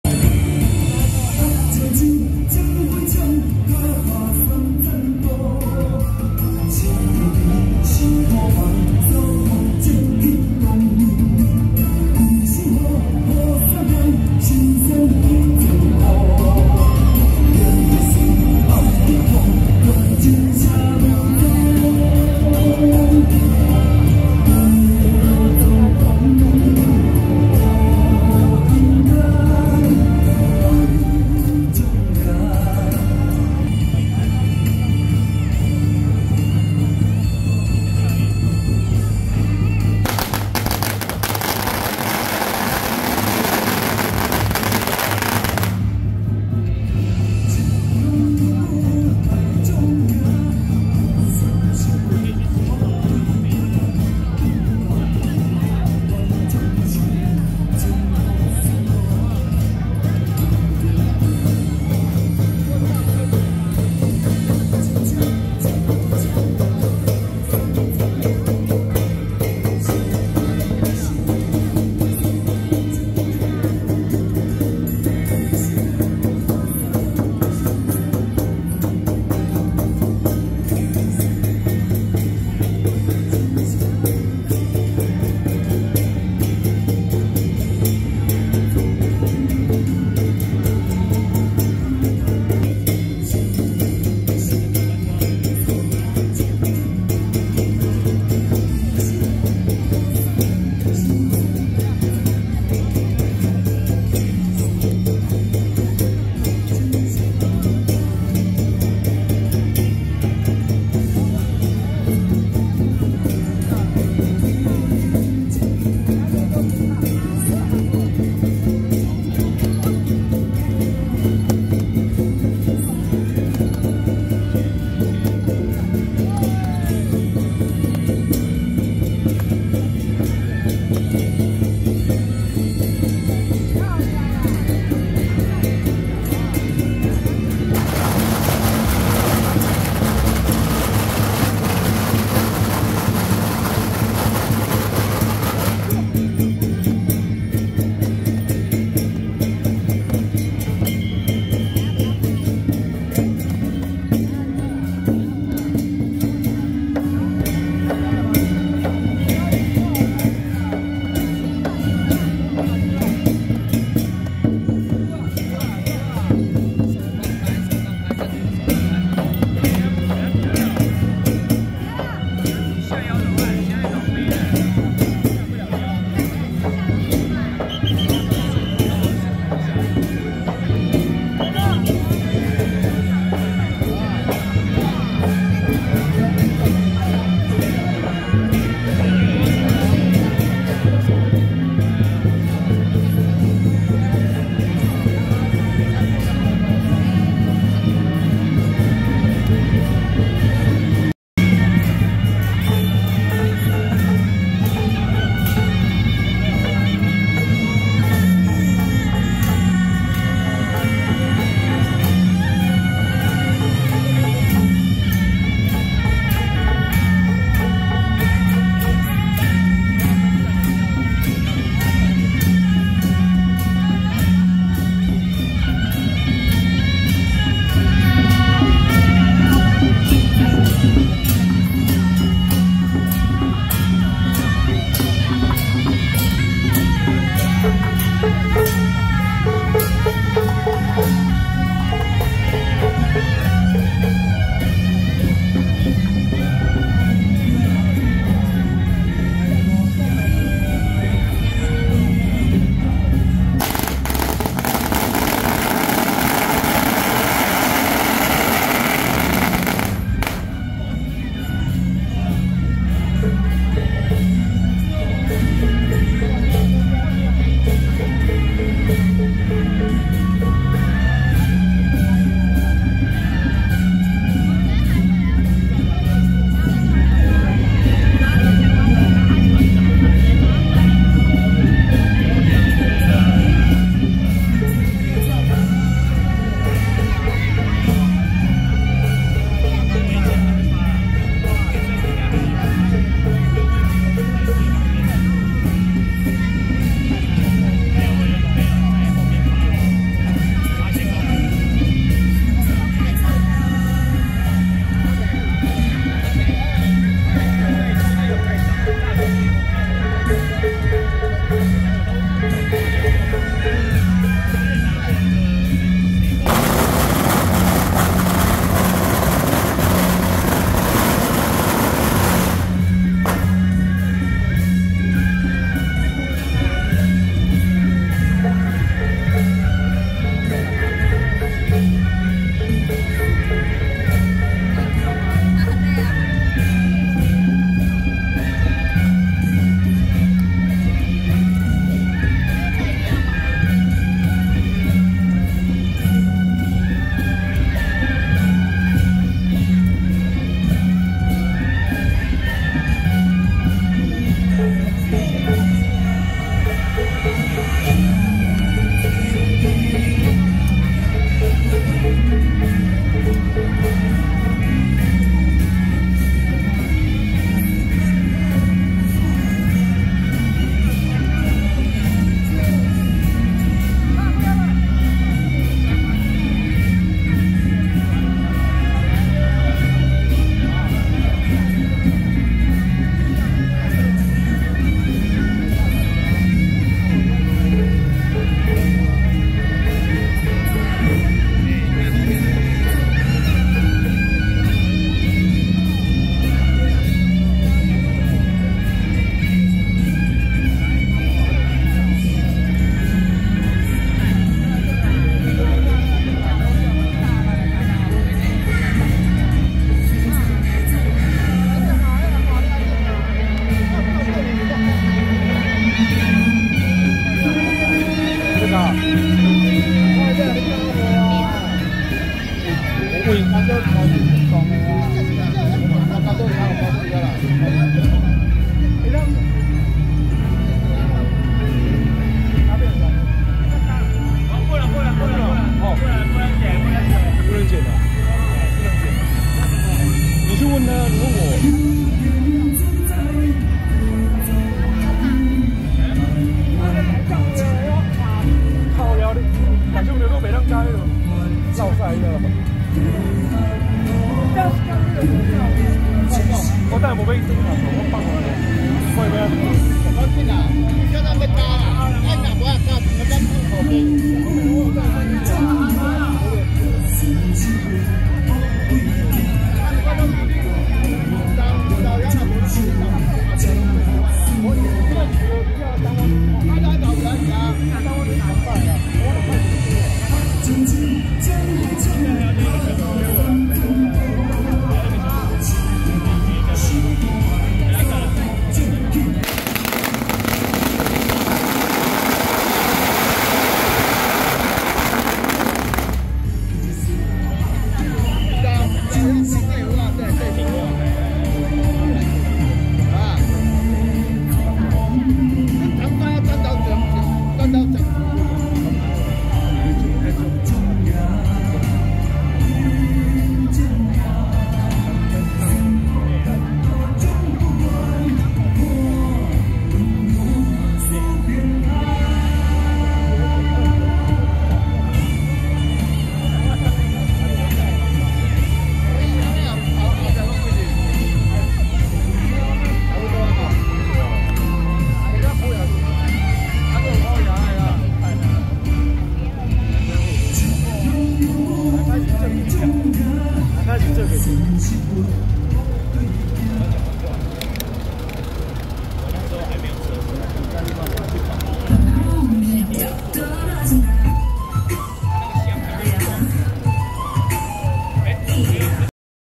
【新莊化成金獅團 慶贊 新莊地藏庵(正日)】~112新莊地藏庵恭祝文武大眾老爺聖誕正日遶境(頭前區)
(0:00)新莊化成金獅團-頭旗西燈車,金獅團
(3:39)泰山貴樂社-北管＆新莊化成金獅團-神將團&達摩祖師 聖駕